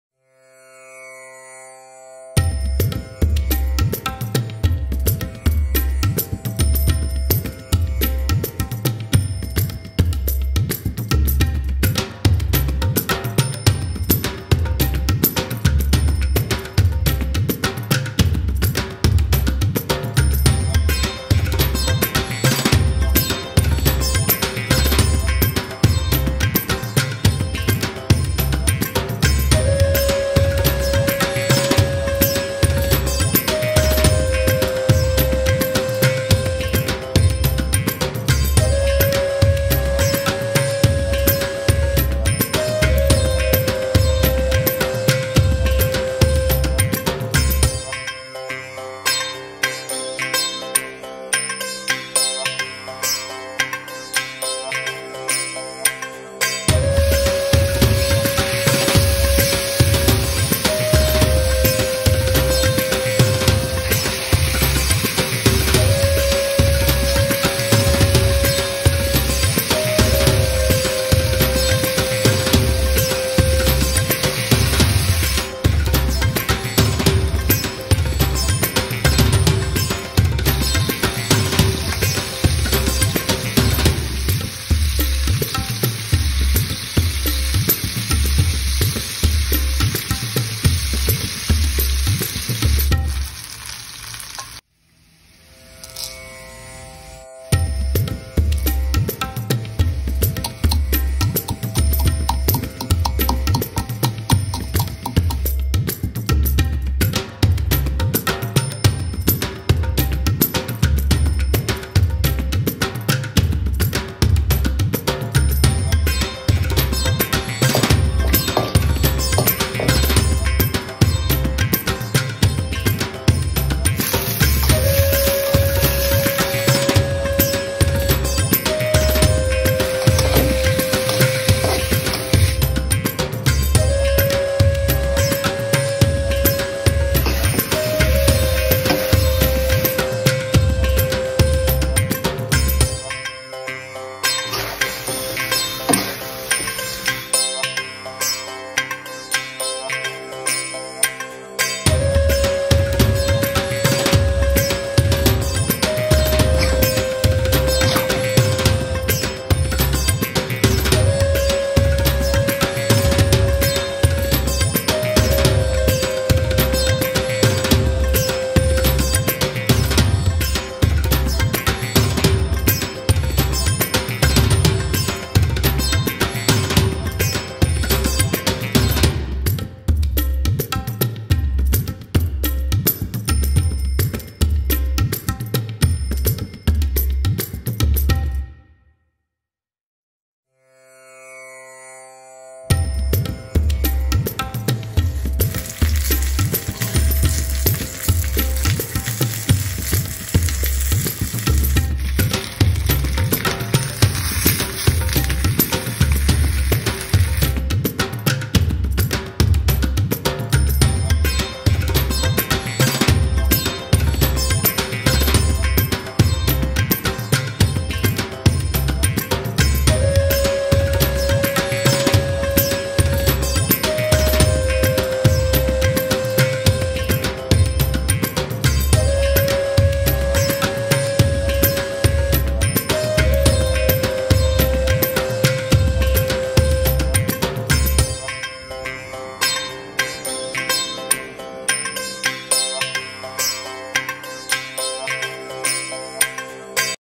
Banana Leaf Chicken Roast [South Indian] (2020)
This is our attempt at doing a banana leaf chicken roast recipe using the traditional Indian way of cooking. Turns out very nice! Do try it at home.